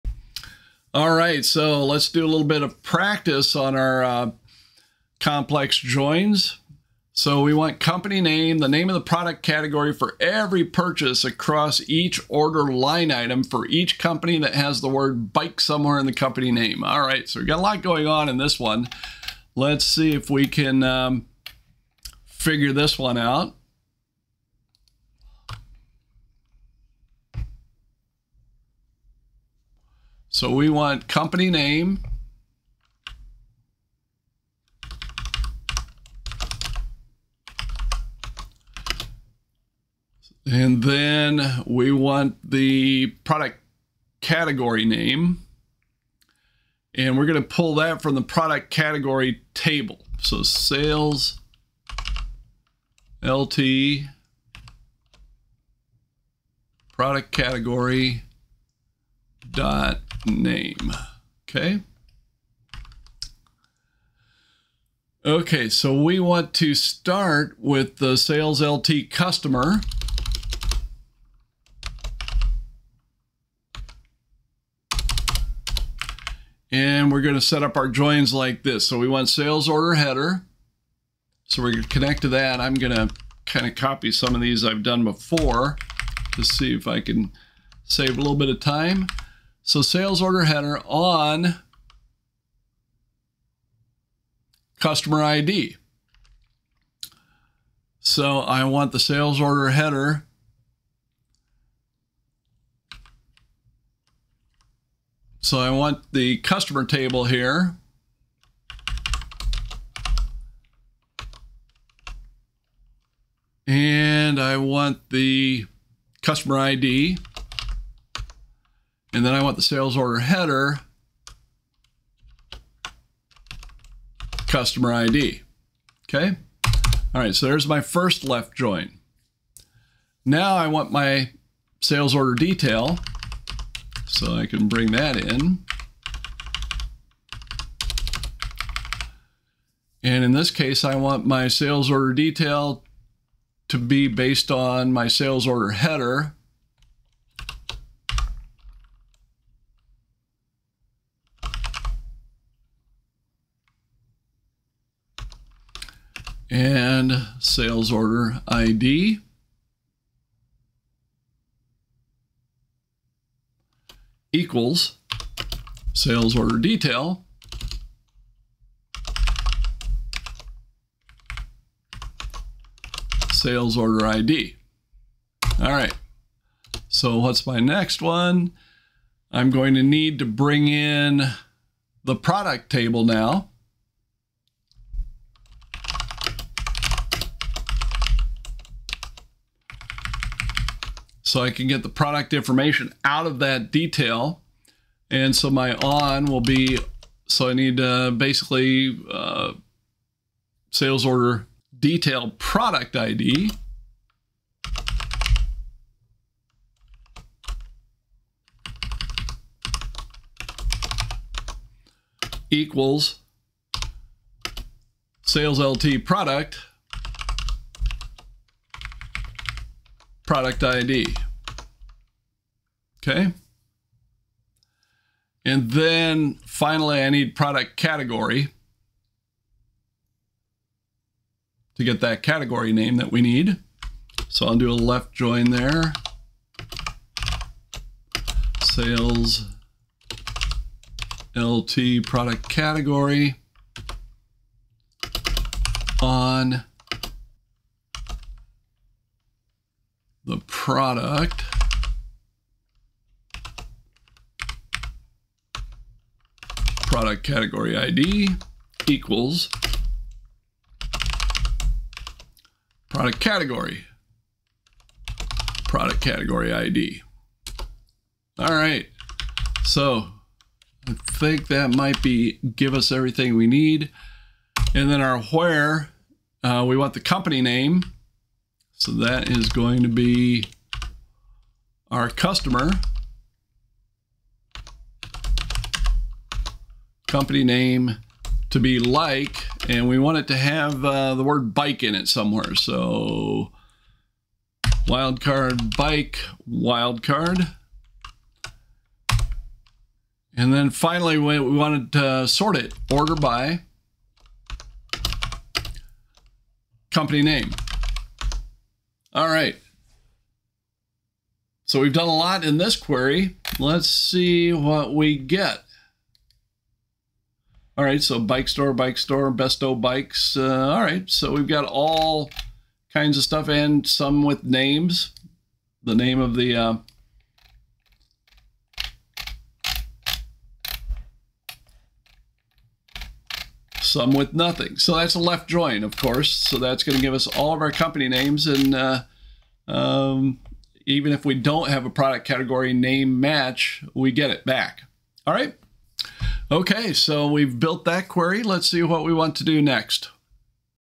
SQL Complex Joins Practice 1
In this walkthrough, we create a SQL query that joins multiple tables and filters the results with a where clause.